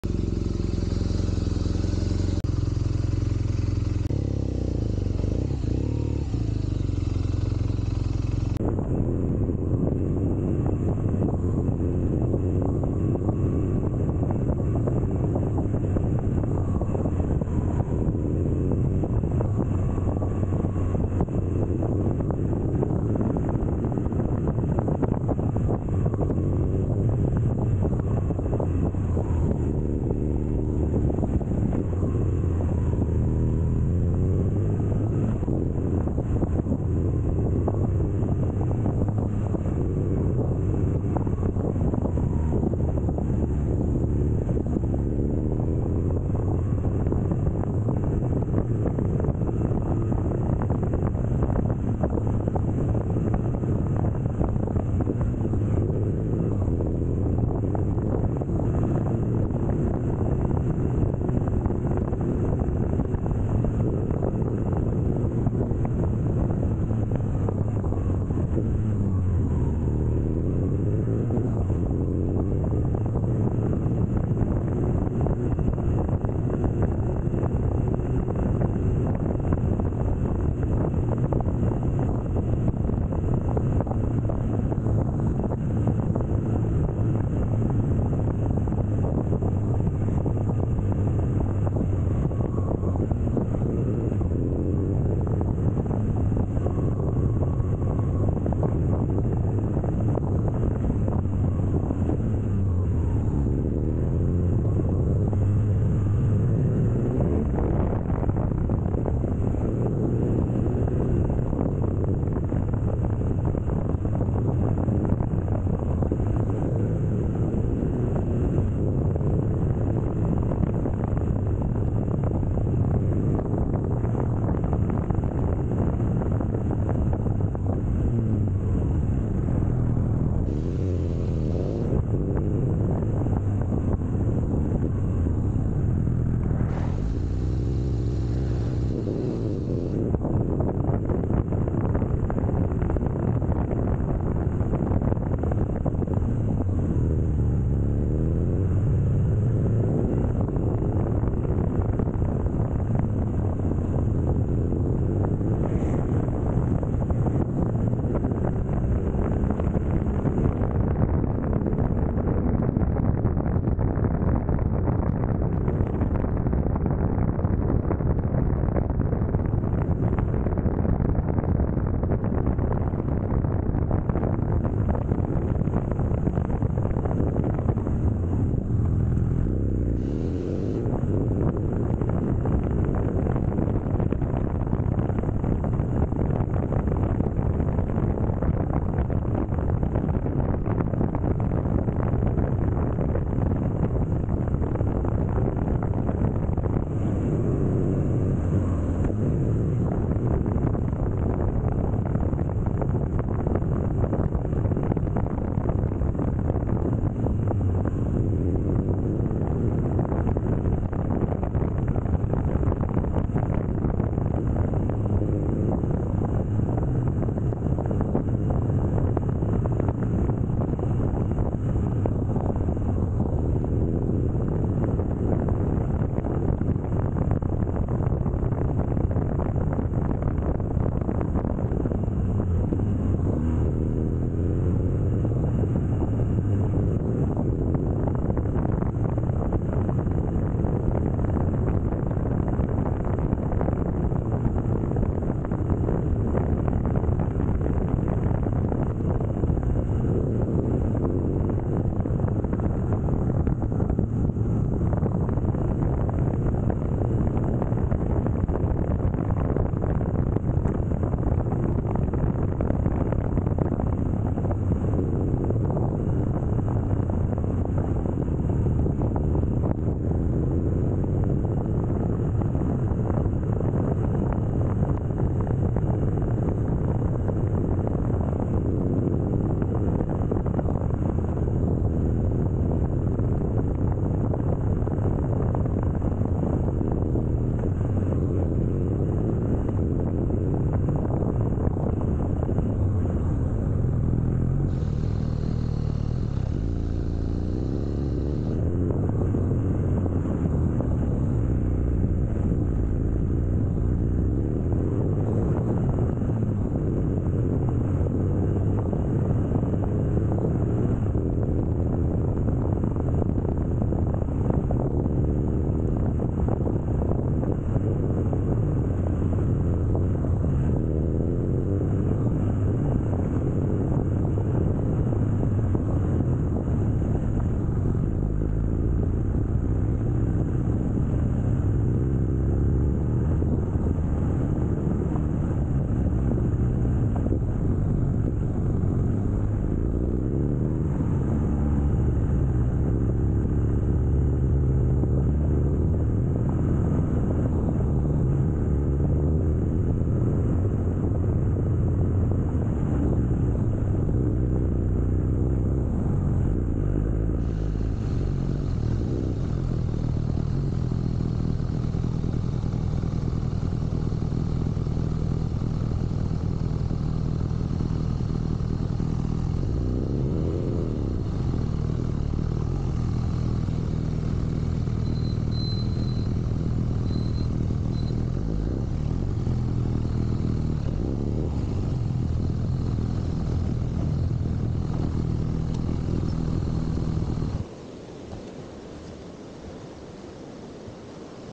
Sunset Ride POV BMW R NineT Scrambler
Riding BMW R NineT Scrambler motorcycle in the evening in Santiago de Chile.

ENJOY MORE FROM OUR CHANNEL!